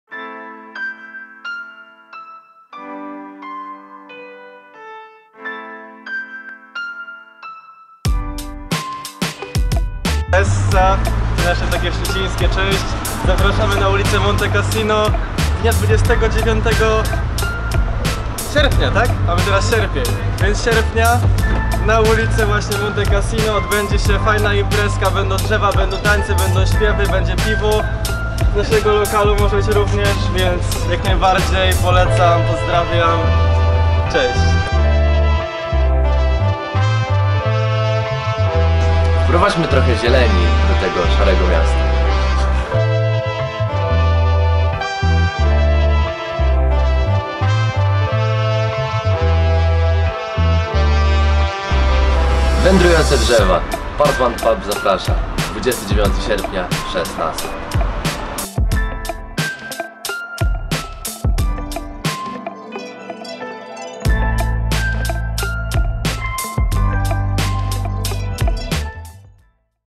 Zapowiedź Wędrujące Drzewa 2020
P'Art One Pub oraz Elysium sklep z dobrym piwem
zapraszają na kolejną edycję Wędrujących Drzew.